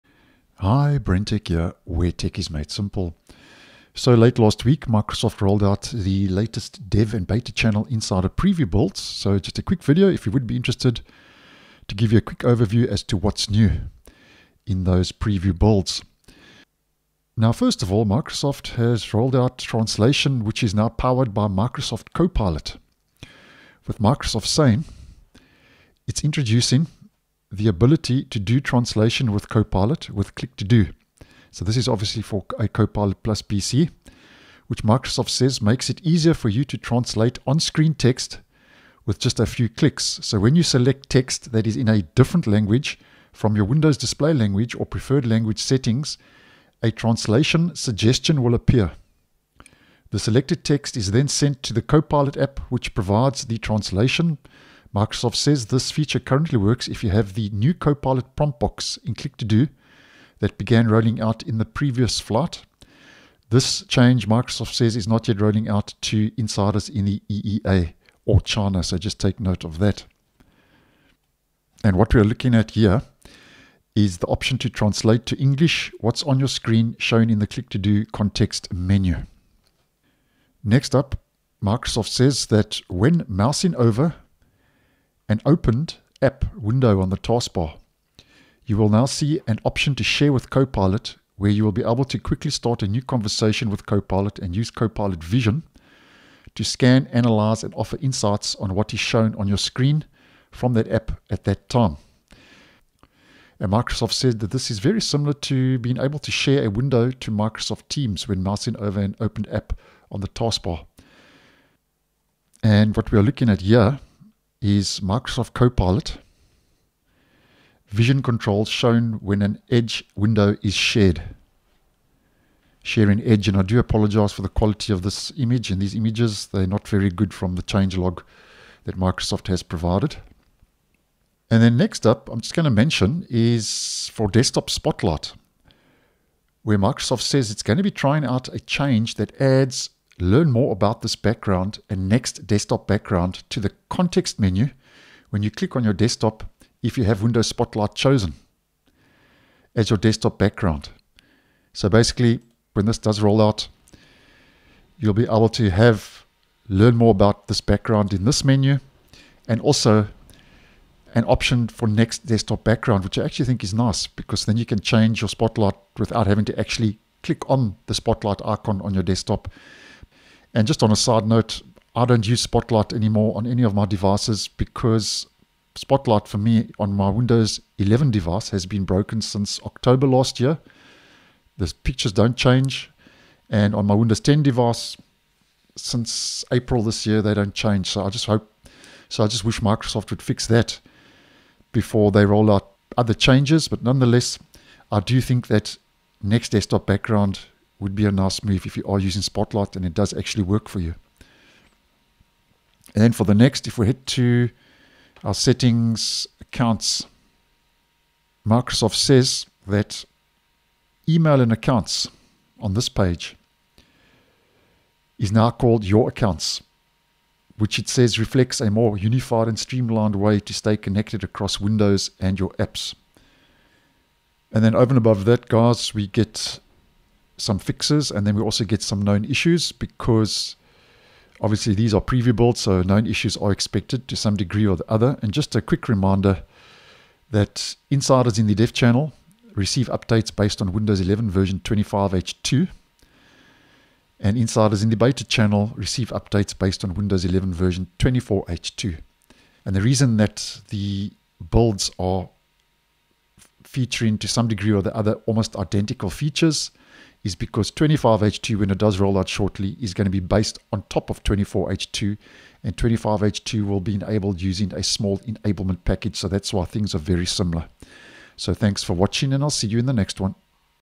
New Windows 11 25H2 Builds in Dev/Beta: What's Coming!
These latest Insider builds include more Copilot, UI changes and more.